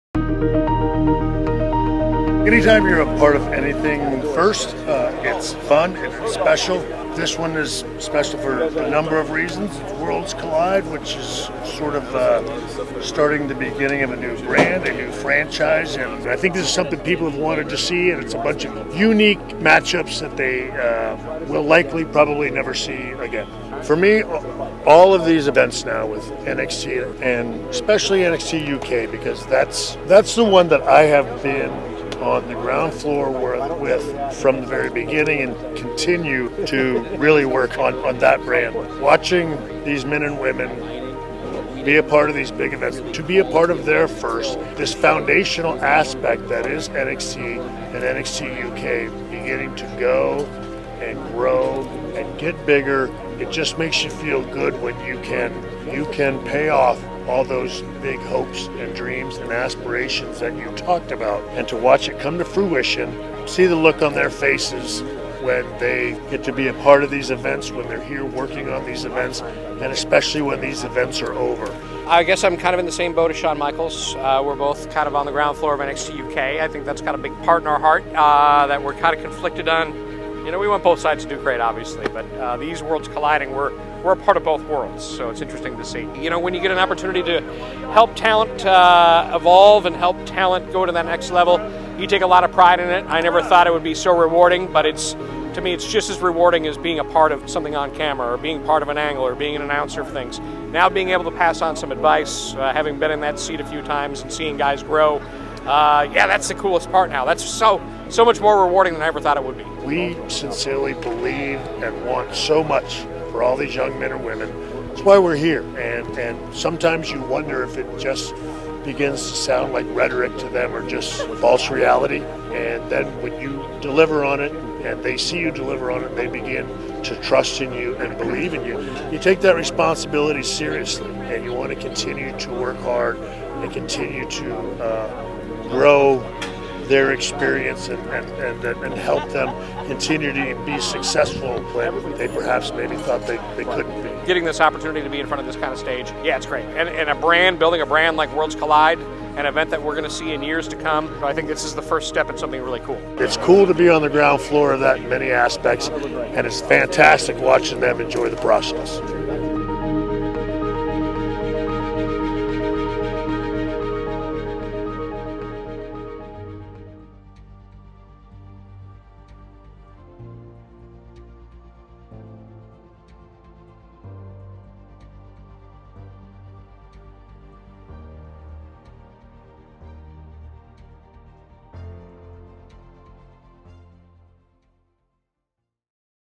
Between Two Worlds | Shawn Michaels
WWE Hall of Famer Shawn Michaels, along with Senior Director of Content and Development, Jeremy Borash, share their thoughts about NXT UK and the development of a new franchise in Worlds Collide.